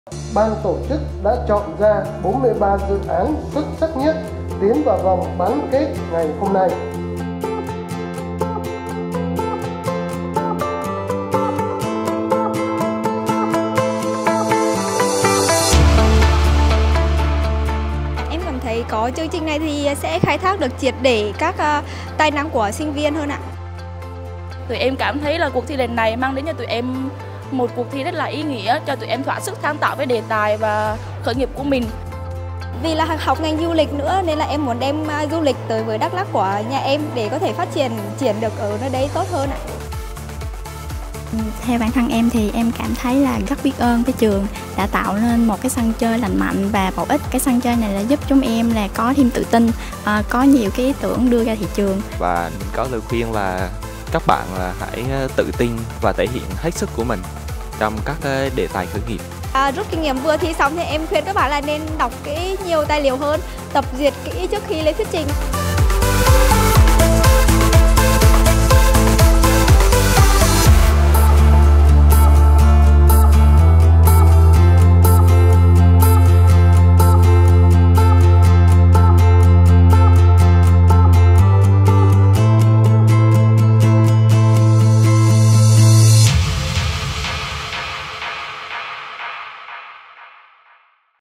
43 dự án sáng tạo tham gia Bán kết UDA Startup Innovation 2024
🎊 UDA Startup Innovation 2024 trong mắt sinh viên là...
... một chương trình hay, khai thác được các tài năng trong sinh viên
... một cuộc thi rất ý nghĩa, sinh viên được thoả sức sáng tạo với đề tài khởi nghiệp ấp ủ
... rất biết ơn trường đã tạo sân chơi lành mạnh và bổ ích, giúp nhiều ý tưởng trong sinh viên được đưa ra thị trường
... cơ hội để thực hiện dự án mang đóng góp phát triển cho quê hương